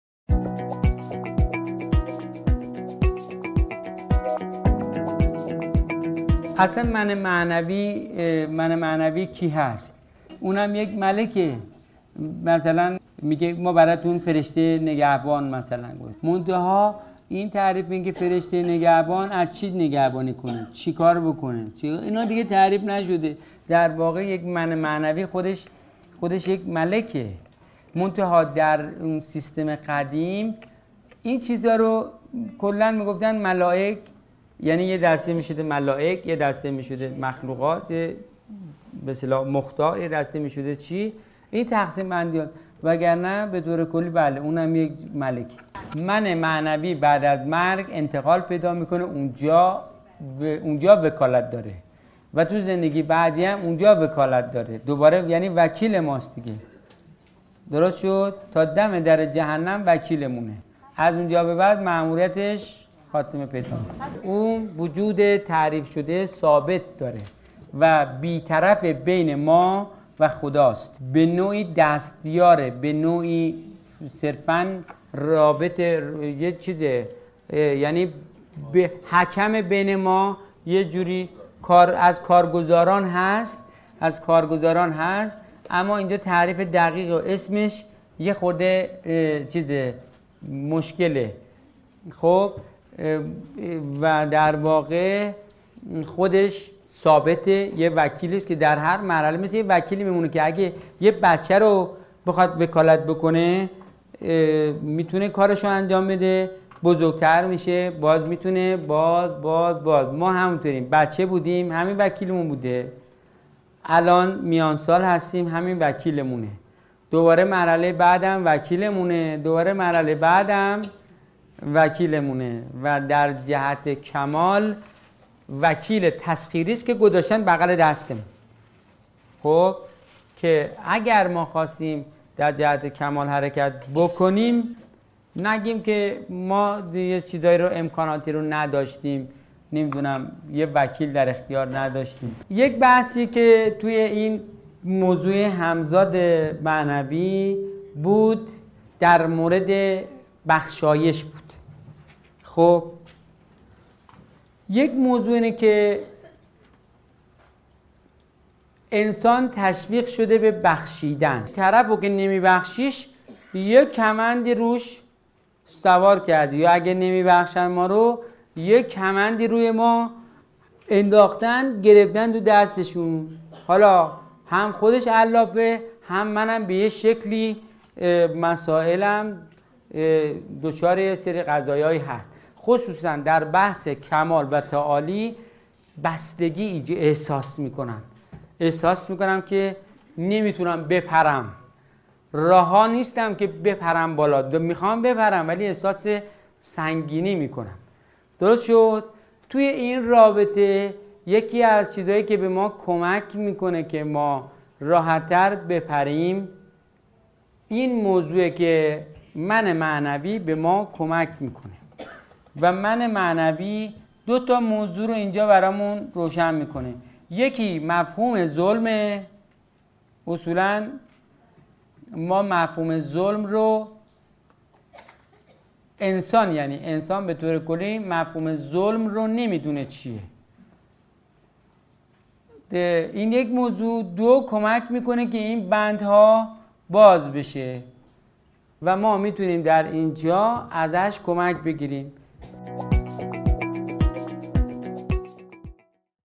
تعریف من معنوی و وظیفه آن در ایجاد بخشش
ویدیوهای دروس موضوعی نظام قدیم عرفان کیهانی حلقه (آکادمی طاهری) از سری دروس ارائه شده در دهه هشتاد خورشیدی توسط محمدعلی طاهری می‌باشد. این ویدیو بخش کوتاهی از جلسه ششم دوره پنج نسخه یک نظام قدیم عرفان کیهانی حلقه است. شما می‌توانید بقیه مباحث مطرح شده در این جلسه را در پلی لیست مربوطه دنبال بفرمایید.